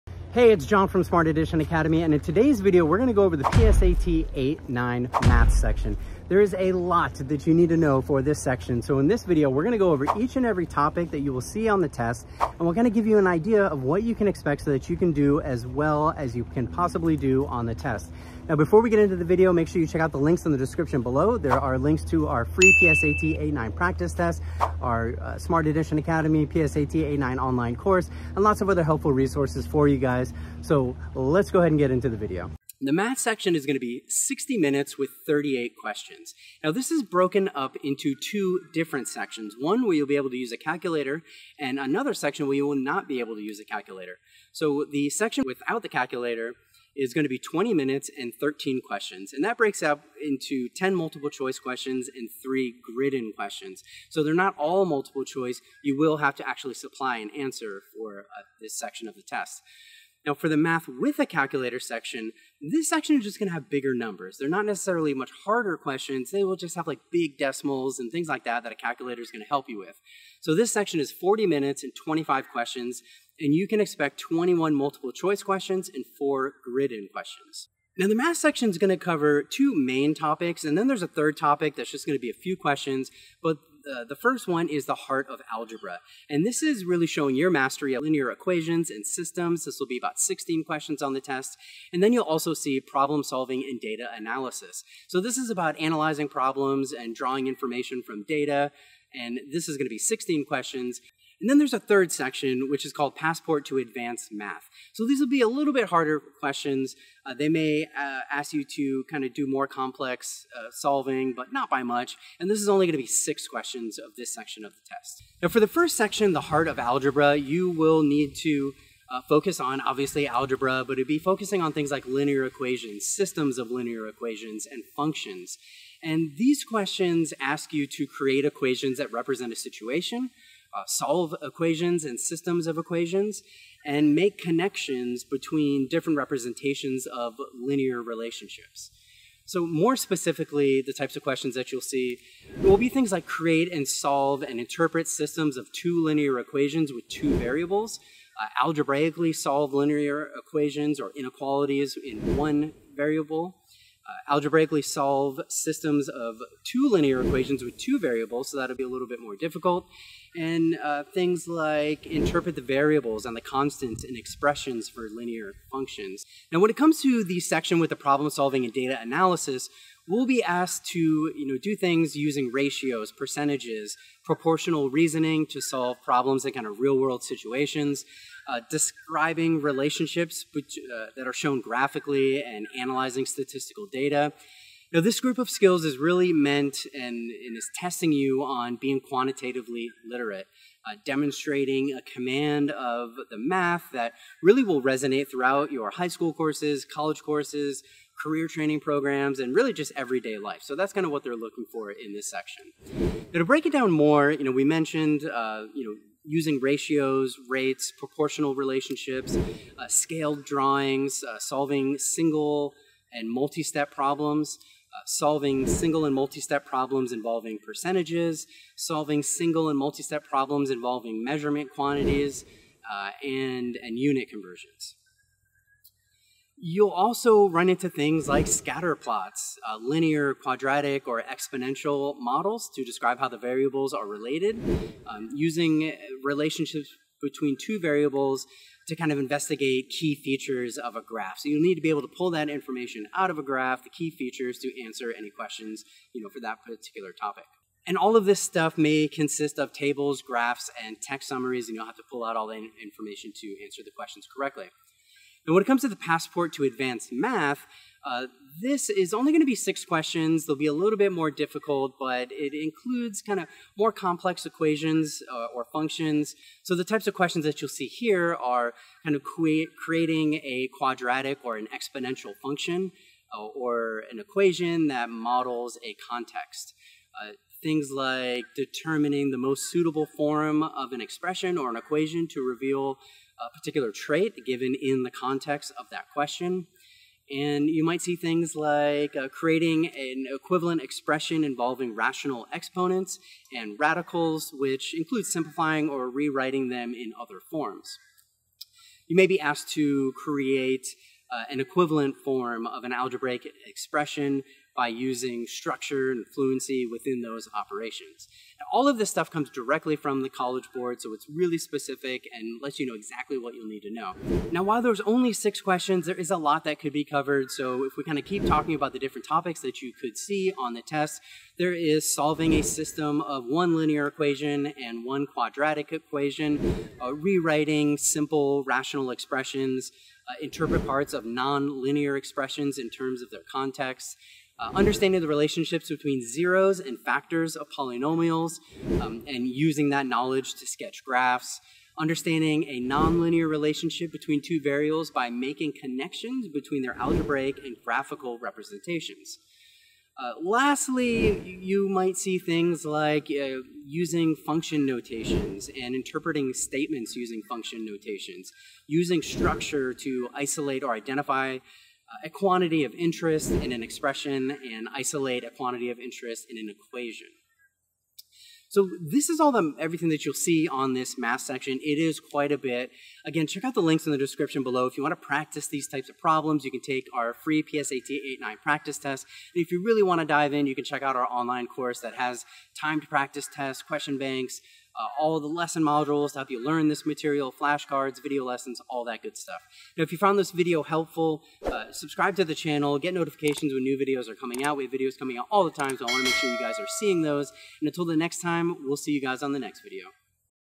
PSAT 8/9 Math 2023 Review
PSAT 8/9 Math Review for 2023 - learn what to expect on the math section of the PSAT 8/9 in 2023.